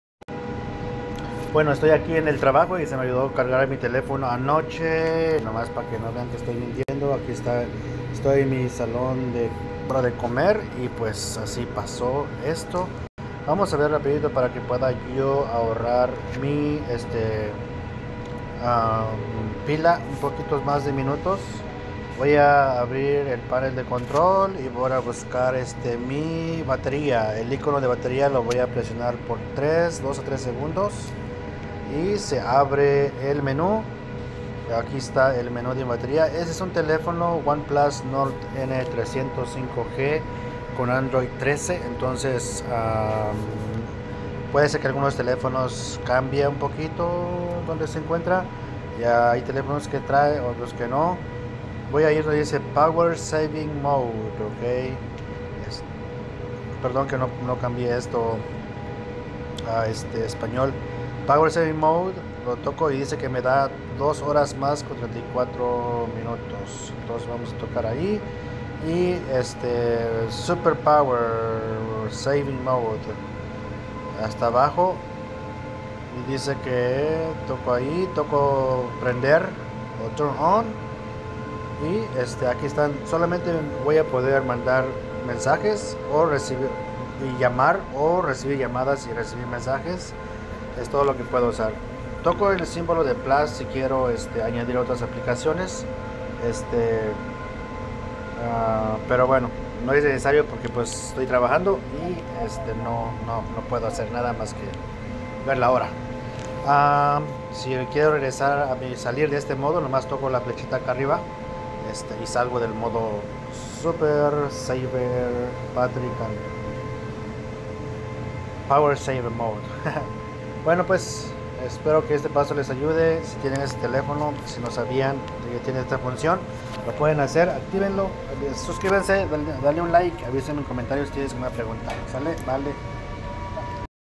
Como aumentar la vida de la batería del celular | Que hacer para que dure más la pila del celular
En este teléfono OnePlus Nord N300 5G trae una opción o función que permite apagar aplicaciones que no se usa con frecuencia para que no gasten la energía de la batería o de la pila y así pueda rendir más , en dado caso que se ocupa ahorrar un poco más de bateria mientras se regresa la luz eléctrica o mientras se consigue un cargador o mientras se llega a la casa para cargarlo.
Esta función de Super Ahorro de Energía lo trae este modelo de teléfono y no es garantía que cualquier otro teléfono lo tenga y de este magnitud.